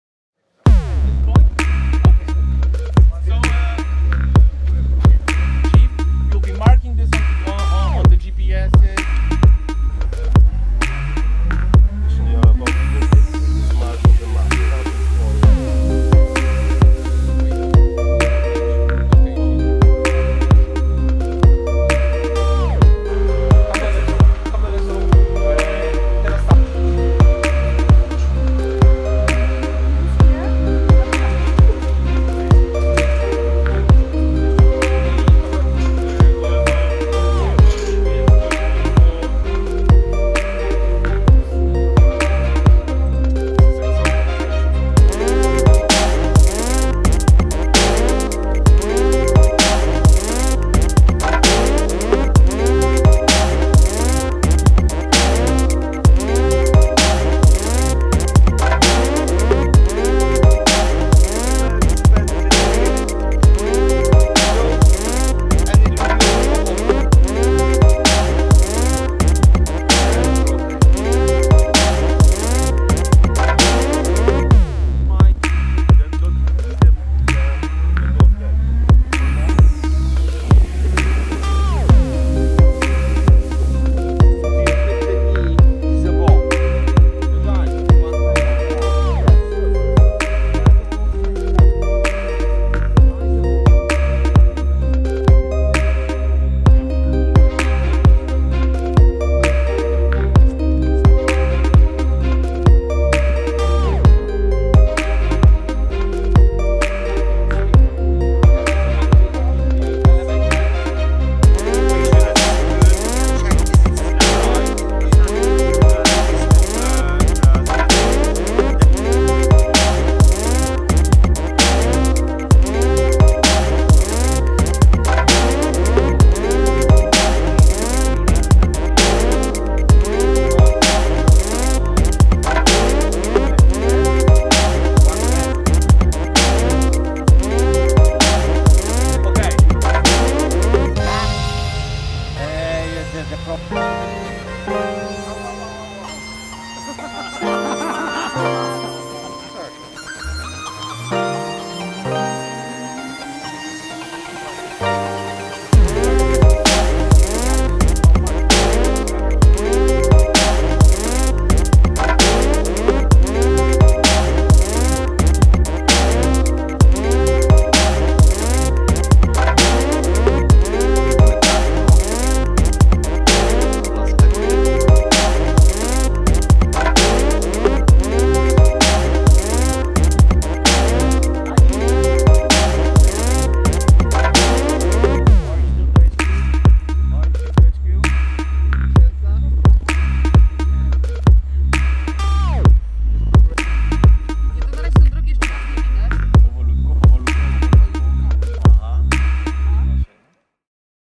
EUPST II - dzień 4 / EUPST II - day 4
Międzynarodowe szkolenie EUPST II. Szkołą Policji w Słupsku. 25 września 2017.
International training of EUPST II. Police School in Slupsk. September 25, 2017.